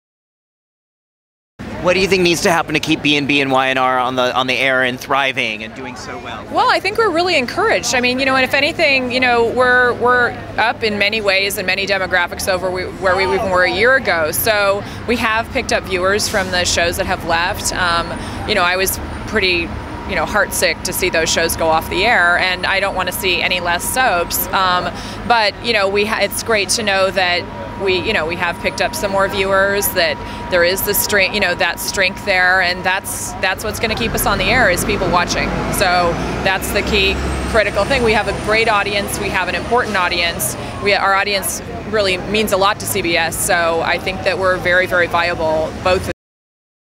THE YOUNG AND THE RESTLESS' Maria Bell: I Don't Want To See Any Less Soaps
WE LOVE SOAPS TV recently caught up with Y&R exec producer/head writer Maria Bell on the red carpet of THE BOLD AND THE BEAUTIFUL's 25th Anniversary party.